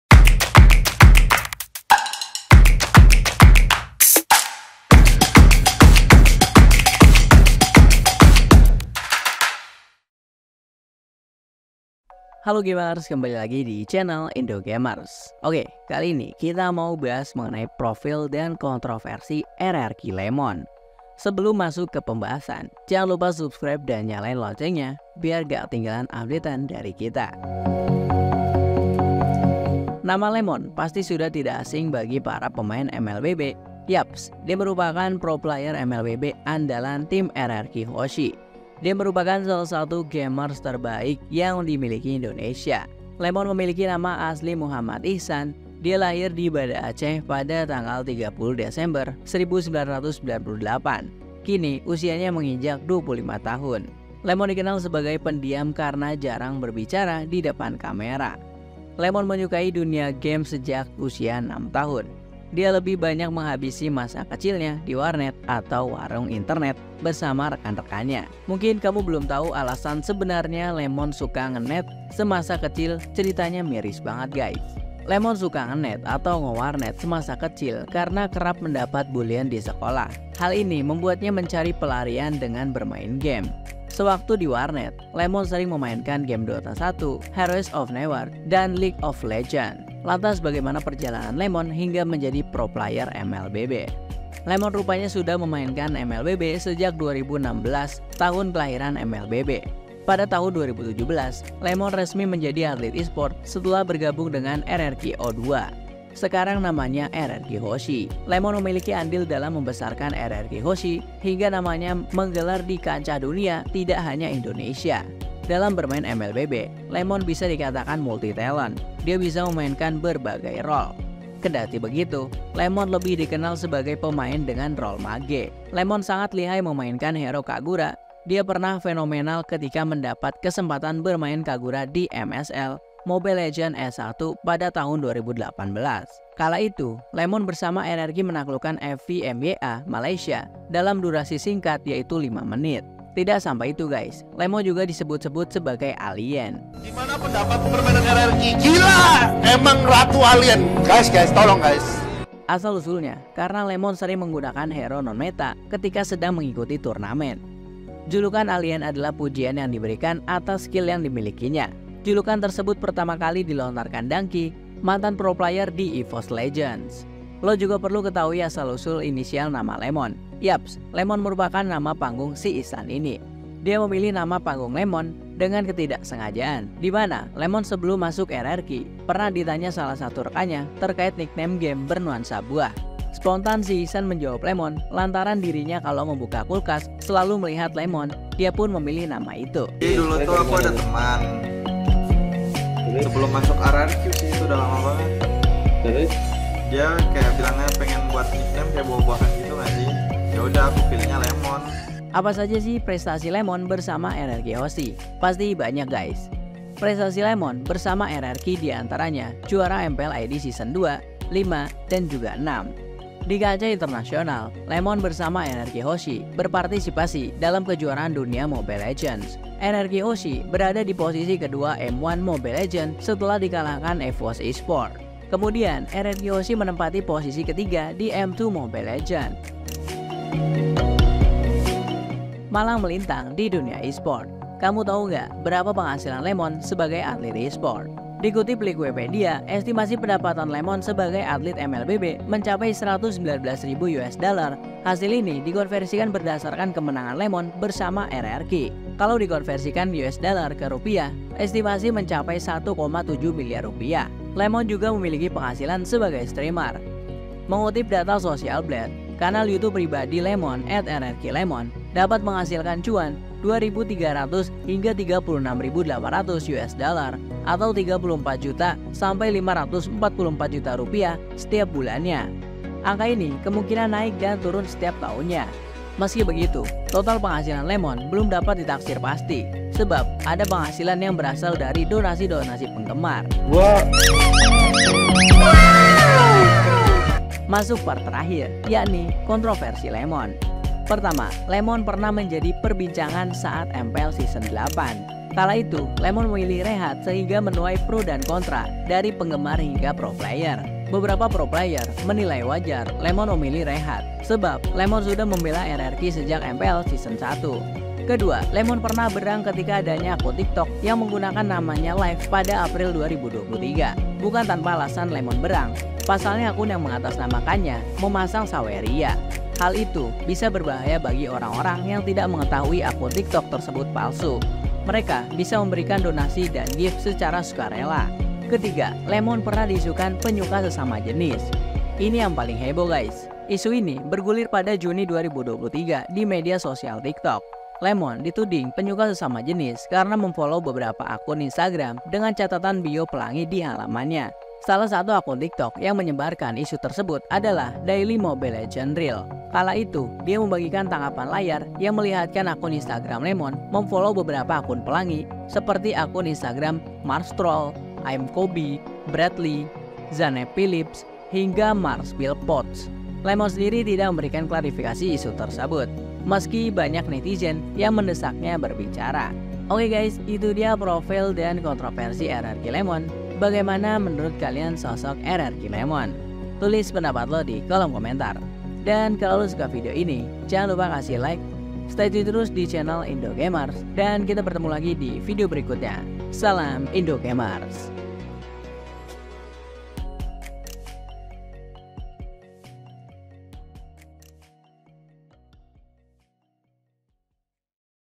Profil dan Kontroversi RRQ Lemon, Salah Satu Gamers MLBB Terbaik Indonesia
Nama Lemon pasti sudah tidak asing bagi para pemain MLBB. Yaps, dia merupakan pro player MLBB andalan tim RRQ Hoshi. Dia merupakan salah satu gamers terbaik yang dimiliki Indonesia.

Lemon memiliki nama asli Muhammad Ikhsan. Dia lahir di Banda Aceh pada 30 Desember 1998, kini usianya menginjak 25 tahun.

Lemon dikenal sebagai pendiam karena jarang bicara di depan kamera.
Lemon menyukai dunia game sejak usia 6 tahun. Dia lebih banyak menghabisi masa kecilnya di warnet (warung internet) bersama rekan-rekannya.

Lemon suka ngenet atau ngewarnet semasa kecil karena kerap mendapat bullyan di sekolah. Hal ini membuatnya mencari pelarian dengan bermain game.

Sewaktu di warnet, Lemon sering memainkan game Dota 1, Heroes of Newerth, dan League of Legends.

Kirim saran atau kritik melalui komen ⬇, DM di Instagram kami @idgsofficial atau sertakan